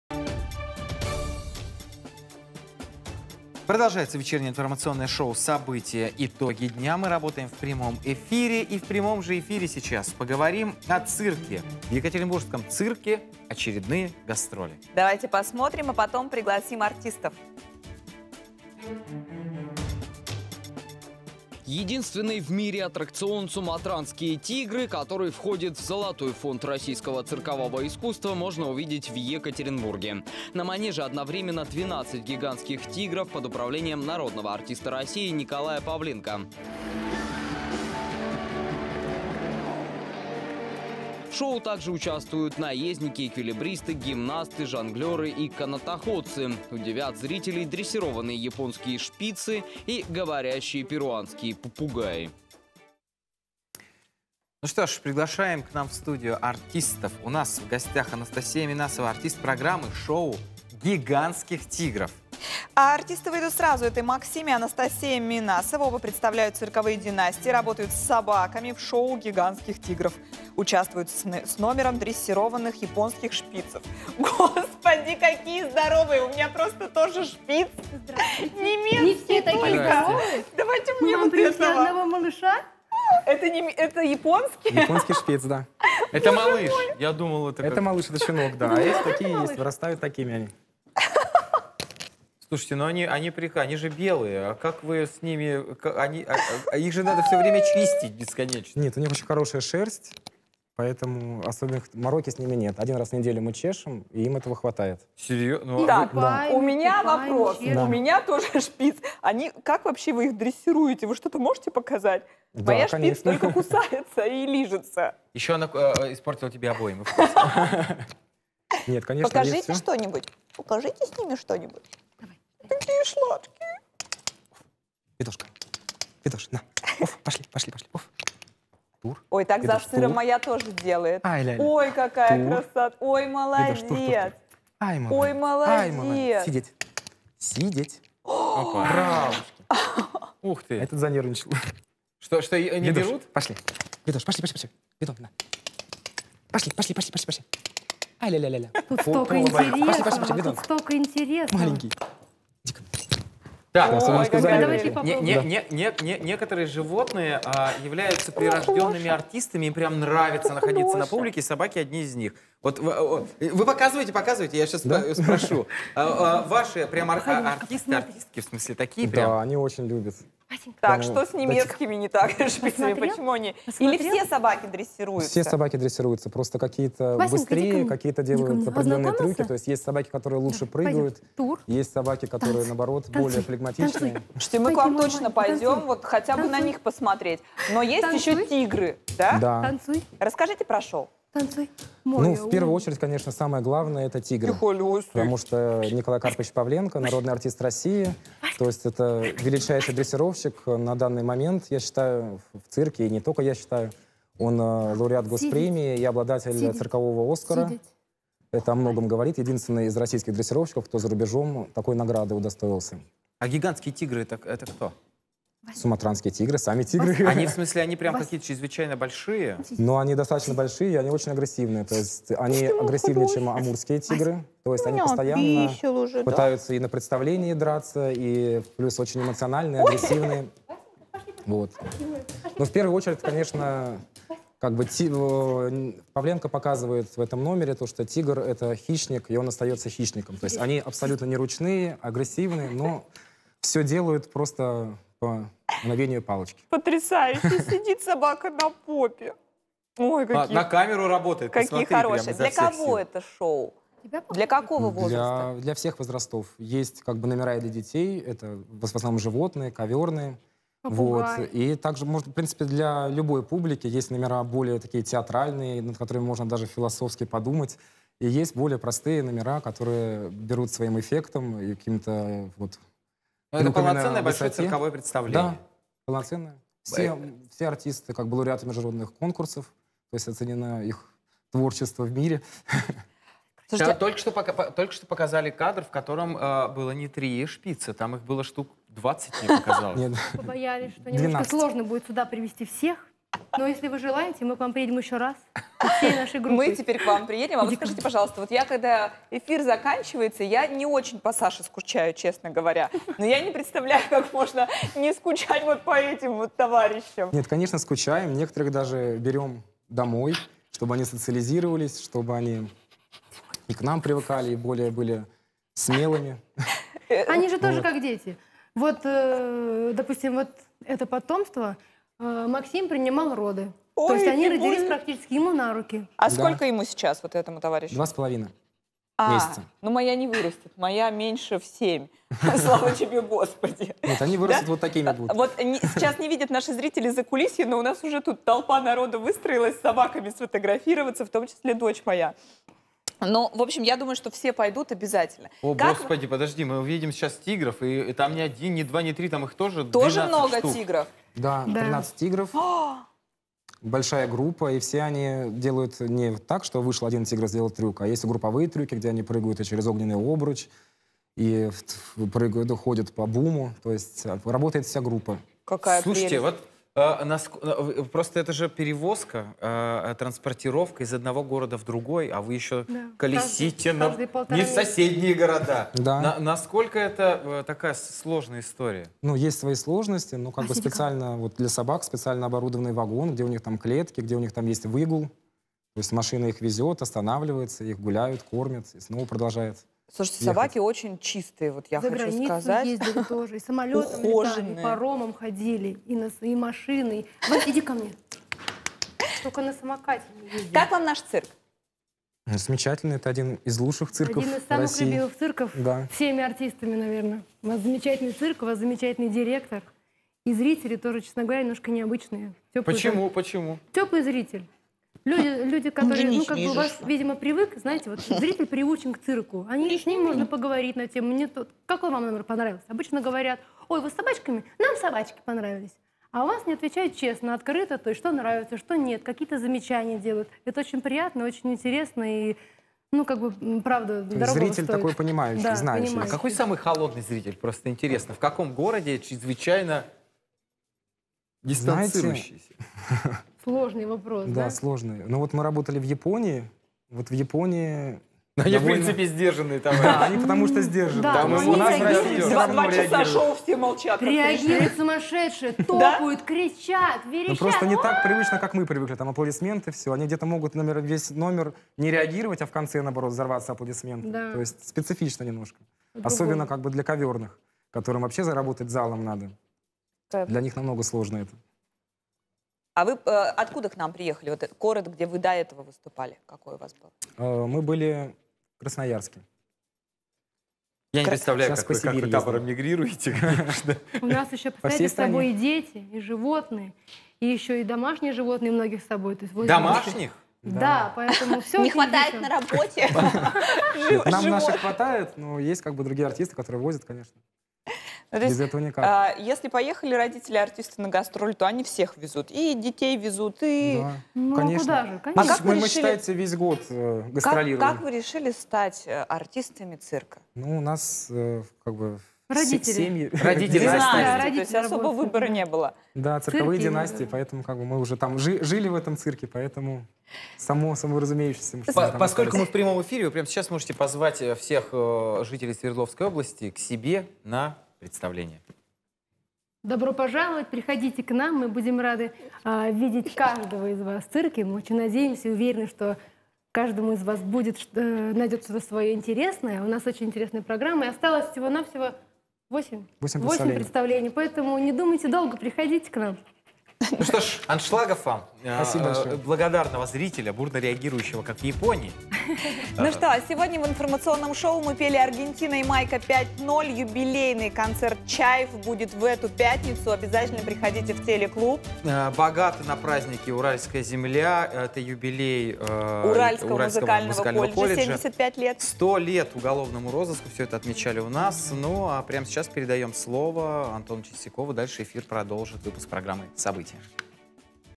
Гигантские тигры в екатеринбургском цирке
В Екатеринбурге можно увидеть единственный в мире аттракцион «Суматранские тигры», который входит в золотой фонд российского циркового искусства. Подробнее о программе шоу рассказывают наши гости – артисты-дрессировщики Анастасия и Максим Минасовы.